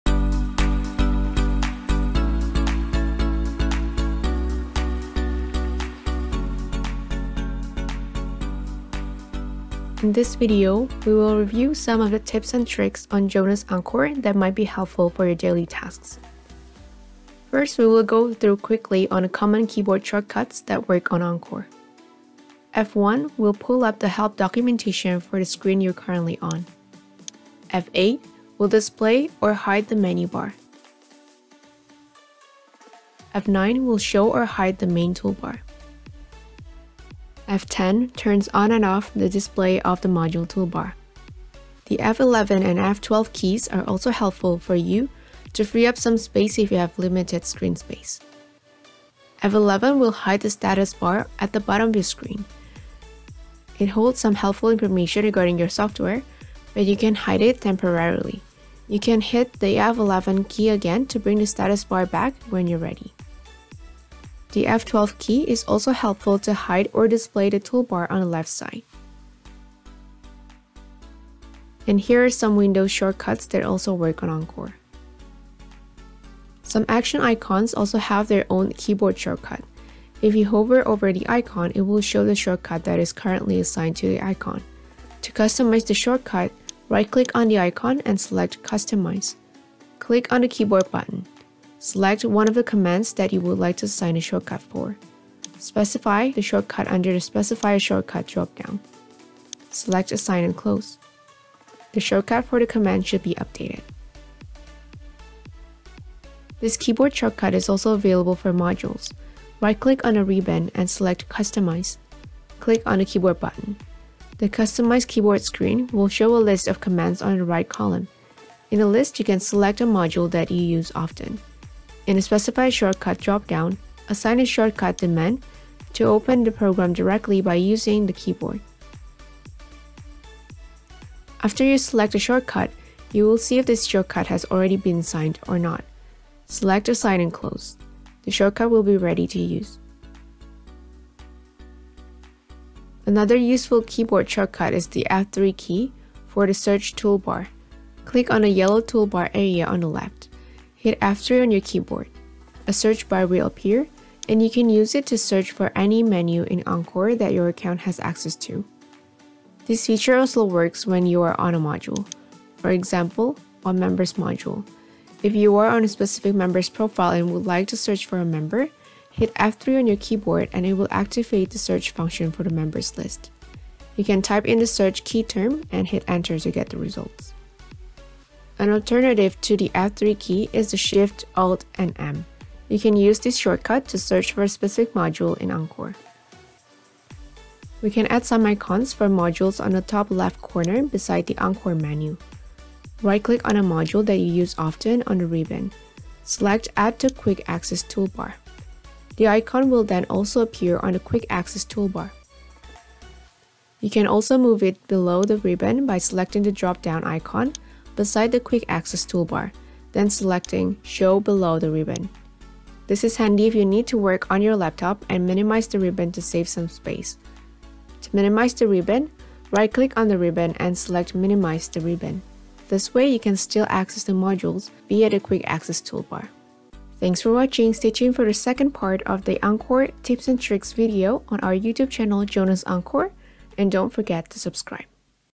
Encore Tips and Tricks - Part 1
This video will review some shortcuts that can be customized within Encore, including how to save some space on your screen when you're working on a smaller screen.

Content:
0:00 Introduction
0:16 Common Keyboard Shortcuts in Encore
1:16 Keyboard Shortcuts for Commands
1:46 Keyboard Shortcuts for Modules
2:22 Module Search Shortcut (F3)
3:08 Quick Access Toolbar
3:37 Minimizing the Ribbon